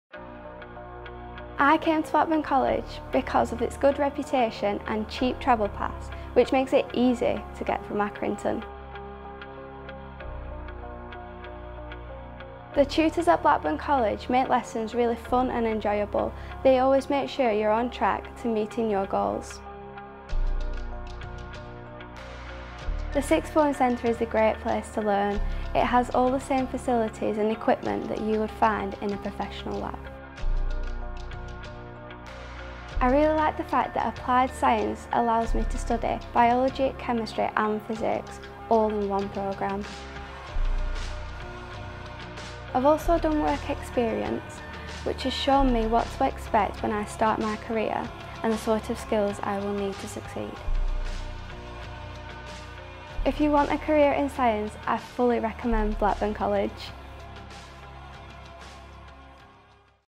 Applied Science courses at Blackburn College
Take part in amazing experiments in our professional laboratories, just like Aimee has! Apply now